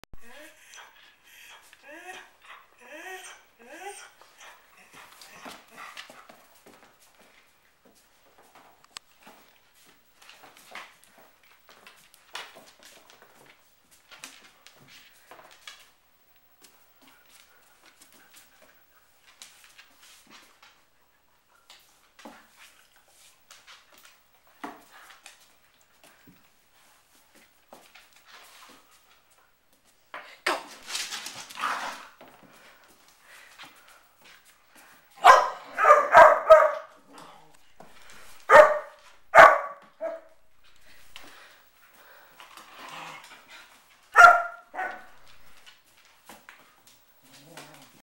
Deerhound puppy taunting Bedlinton terrier
Our 3 y/o Beddie, gizmo, does NOT like it when his new (and to him) annoying little brother (Sirius, a 4 mo old Scottish Deerhound) plays with his toys. Just like children, there is a competitive rivalry going. What I love about this vid is the expression on his face before Sirius gets his toy, and then the expression when it pans back. Gizmo quickly sets his brother straight when he's allowed. It's all in good fun!